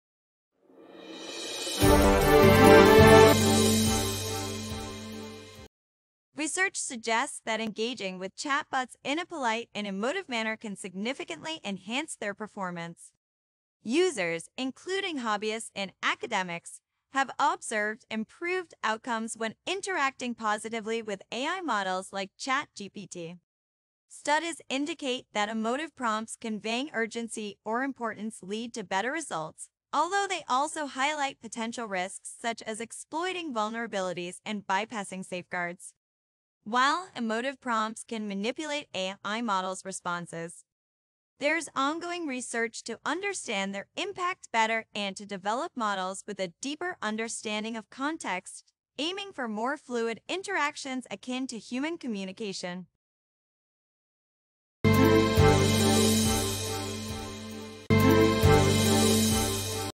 Treating Chatbots Nicely Enhances Performance: Insights and Implications
Research suggests that engaging with chatbots in a polite and emotive manner can significantly enhance their performance. Users, including hobbyists and academics, have observed improved outcomes when interacting positively with AI models like ChatGPT. Studies indicate that emotive prompts conveying urgency or importance lead to better results, although they also highlight potential risks such as exploiting vulnerabilities and bypassing safeguards. While emotive prompts can manipulate AI models' responses, there's ongoing research to understand their impact better and to develop models with a deeper understanding of context, aiming for more fluid interactions akin to human communication.